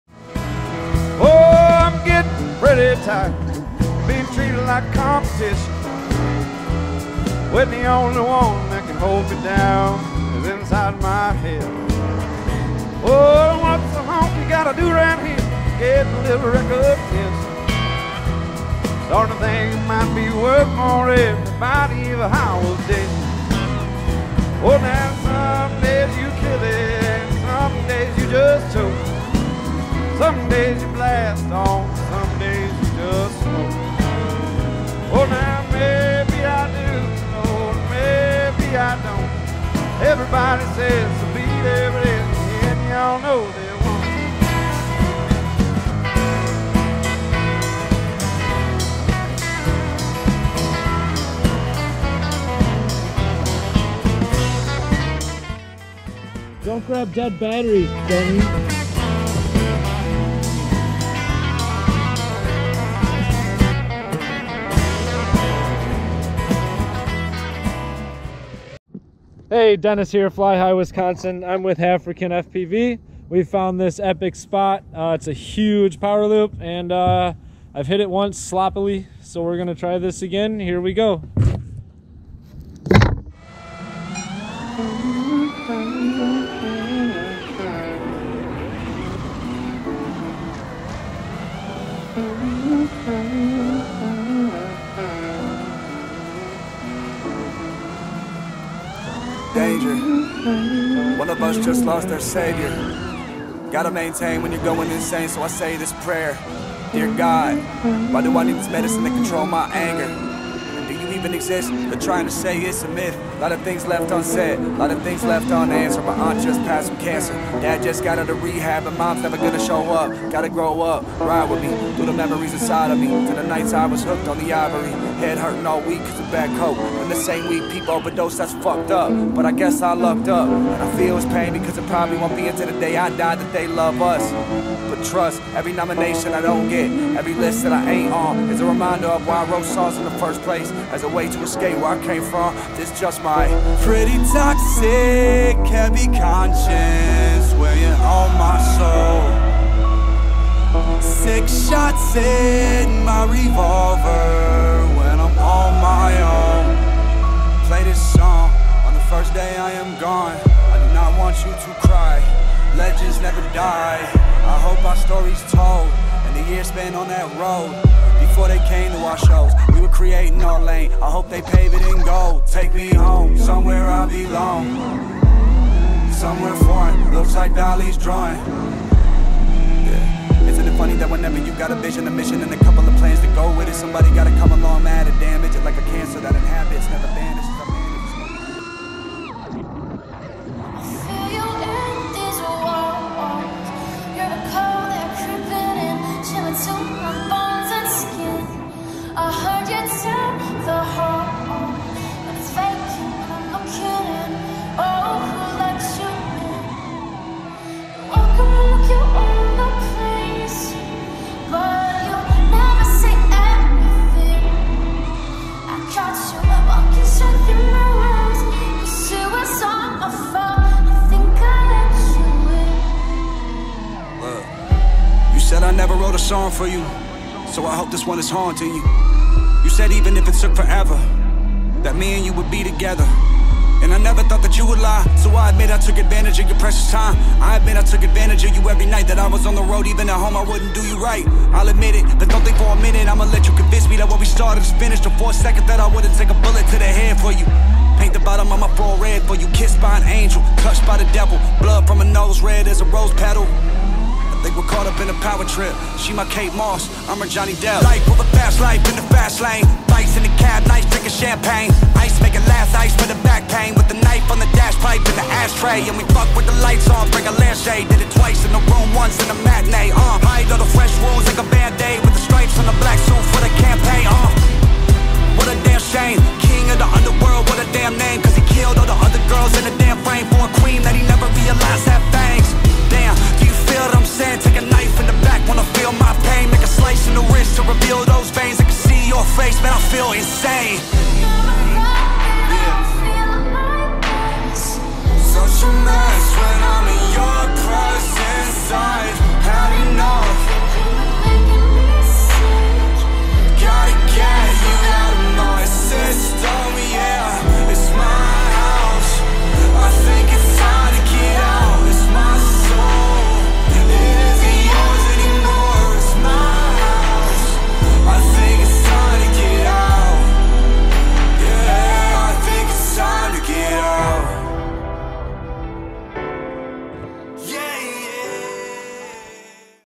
New Spots, New Flow...
Found a couple new spots with HalfricanFPV! Hope you enjoy! Summer is here and things are heating up here in Wisconsin!!

Gear:
Fatshark HDO w/ RapidFire
Radiomaster TX16S w/ TBS Tracer

BQE Bot5
T-Motor F7 FC w/ 45A ESC
TBS Grinder V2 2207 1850kv Motors (6s)
Foxeer T-Rex
Lumenier Mini VTX
TBS Tracer Nano RX
True RC LED Long Antenna
Ummagawd 4 Play Props

BQE BOTAFX Banger
Foxeer T-Rex
Rush Tank Mini VTX
True RC Long Antenna
GEMFAM Hurricane X Props